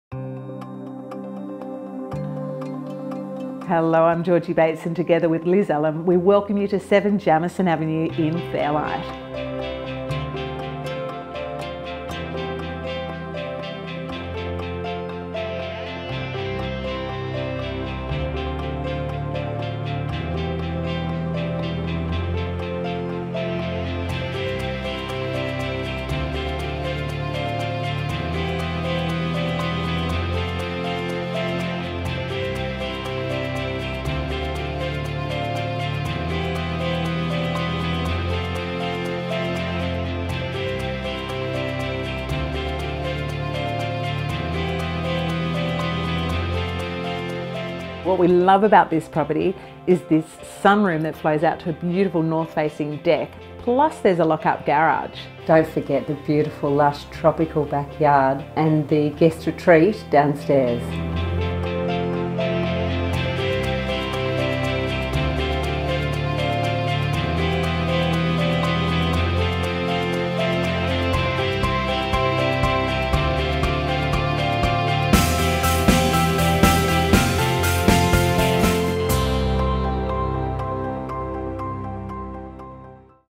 SOLD | 7 Jamieson Avenue, Fairlight│Georgi Bates│Elizabeth Allom│Cunninghams
Family Delight in the Heart of Fairlight

Deceptively spacious and exceptionally versatile, this charming and character-filled semi offers low-maintenance living in an exceptionally family-friendly setting.

With a perfect northerly aspect in one of Fairlight's premier streets, it exudes a lovely relaxed ambience and offers a versatile layout across multiple living and bonus spaces.

Inside, the interiors serve as a gracious reminder of the home's heritage. Period features include lofty ceilings dressed in ornate plasterwork, timber ceiling beams, sash windows, arched doors and original timber polished floorboards, creating an unmistakable aura of space and breeziness throughout. Pendant lighting and plantation shutters add clever contemporary touches to this home and serve as a small taster when it comes to the possibility of further enhancements.

Down a breezy hallway servicing two generous bedrooms and a free-flowing casual living and dining room to the rear, the main floor of this home offers wonderful versatility for family living.  The addition of a sunroom at the fore of the home is a bright bonus, as is the terrace adjoined to it, delivering dreamy district views and northeasterly breezes, which flow throughout to fill this home. Whether it's a morning cuppa or an evening sundowner, this terrace will beckon you.

The expansive guest or grandparents retreat situated on the lower level, offers private seclusion with its own spacious ensuite, while the upper loft level offers a surprise space with fabulous flexibility to use as teen retreat, rumpus room or home office. The potential to expand this space (STCA) to create further accommodation presents an exceptional proposition.

A blissfull Bali inspired backyard featuring an inbuilt BBQ area showcases landscaped gardens and leafy trees, providing the perfect amount of shading and privacy for kids to play and parents to entertain with ease.

Additional features include an oversized kitchen, single lock-up garage with mezzanine storage, enormous under house dry storage area that offers wine cellar and workshop space, a large main bathroom with separate shower and bath as well as dedicated ironing and laundry rooms.

Within walking distance to Fairlight Village where cafes, boutiques and restaurants are all counted as neighbours and with easy access to buses and Manly Beach, this is the perfect postcode!

Land size - 271.9 approx.